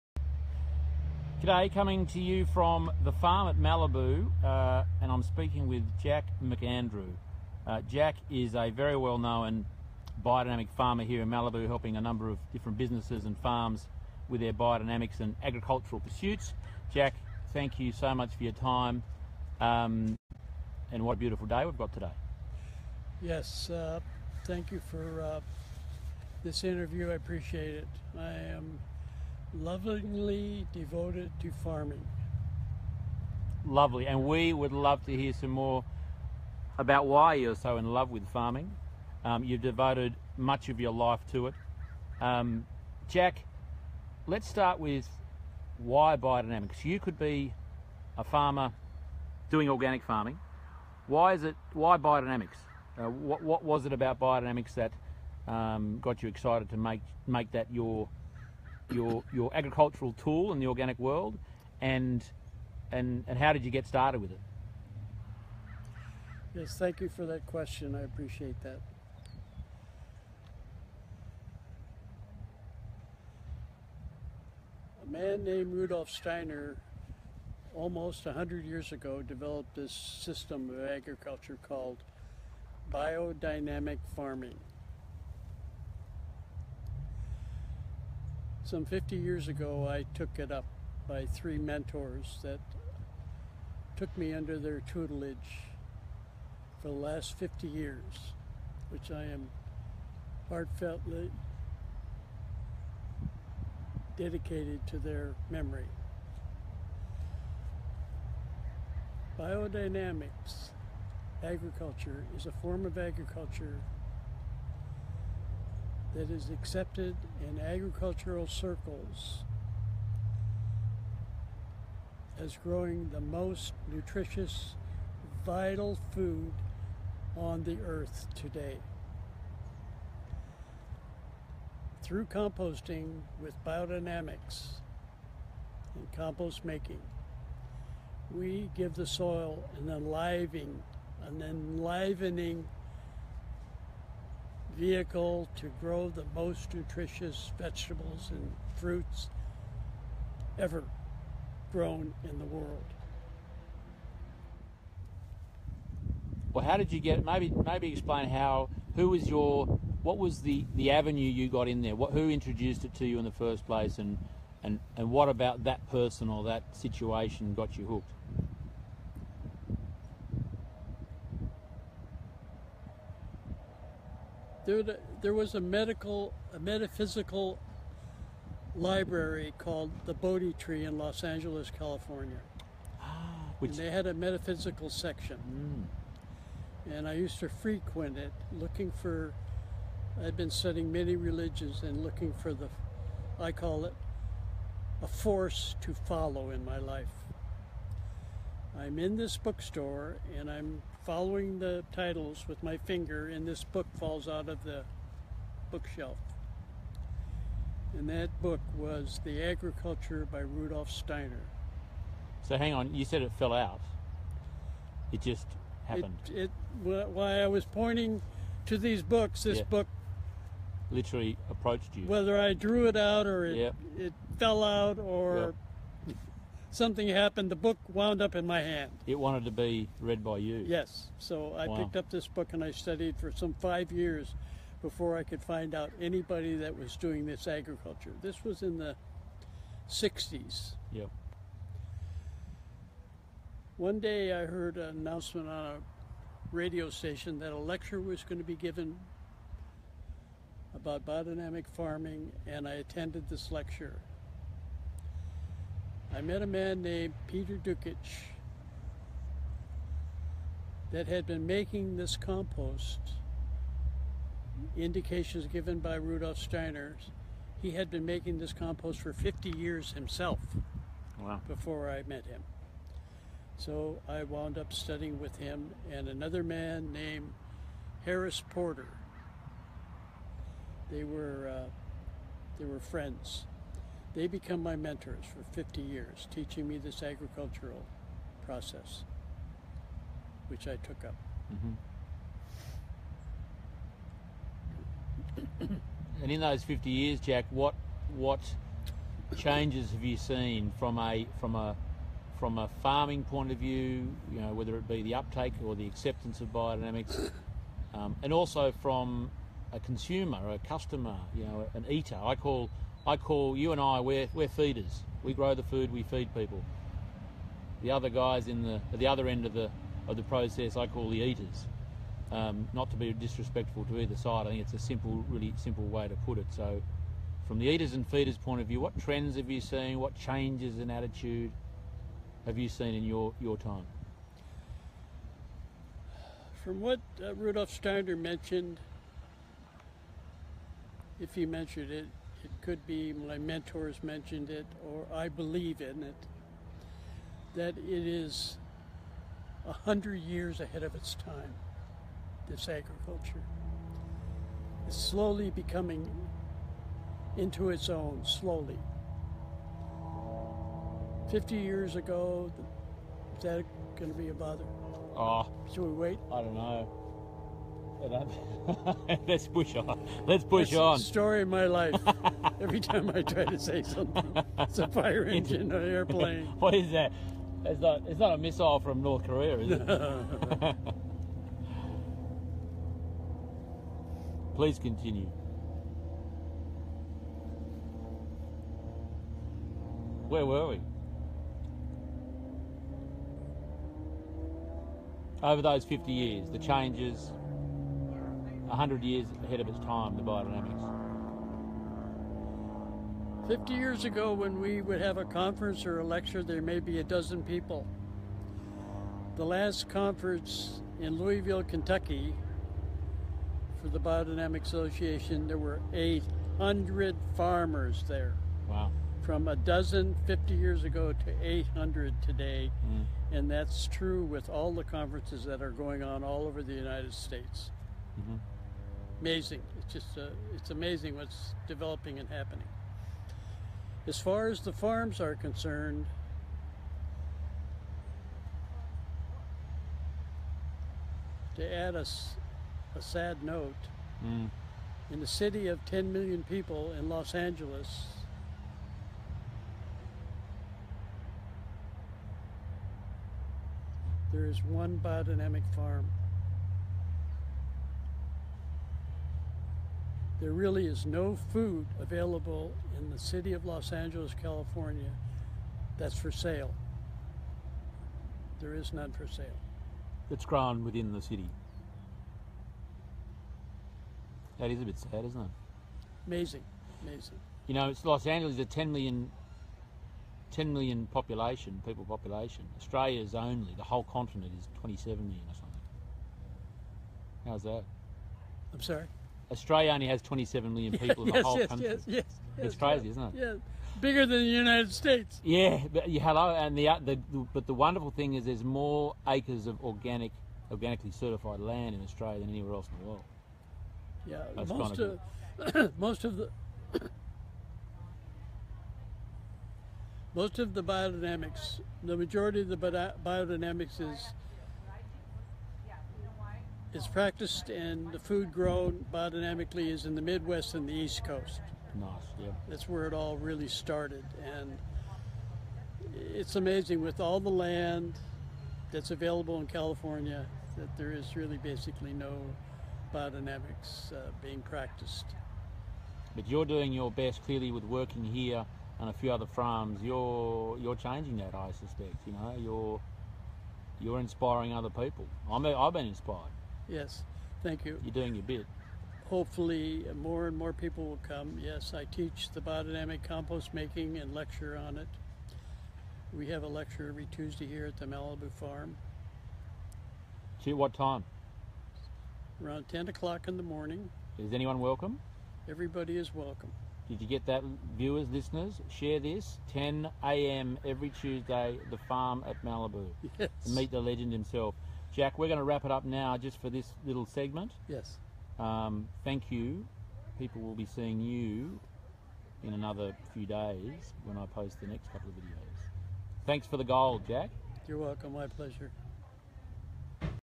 Biodynamic Farmer Jack McAndrew from The Farm, Malibu.
Lovingly devoted to farming……..legend Biodynamic sage and farmer Jack McAndrew has been practicing Biodynamics for more than 50 years, and he explains how BD grows the most ‘nutritious vital food on the earth today’, how he found this ‘force to follow in his life’, and the mentors that played such a pivotal role I his understanding of BD. If you want to meet Jack in person you can catch up with him at 10am everyday at The Farm at Malibu for lectures and practical experience.